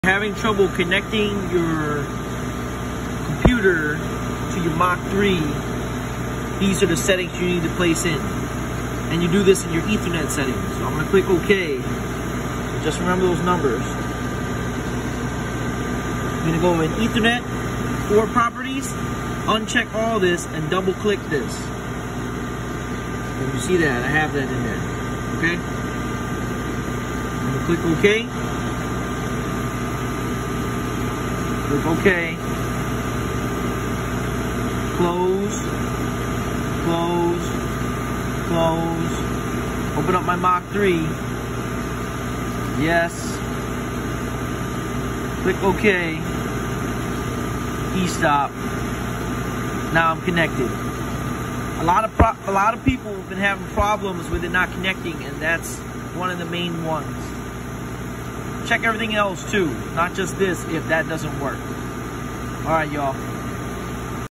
Troubleshooting why Mach 3 isn’t connecting to your computer | FastCut fastcutcnc
If you’re having trouble connecting your Mach 3 to your computer then this is one of the steps you can use to fix that issue.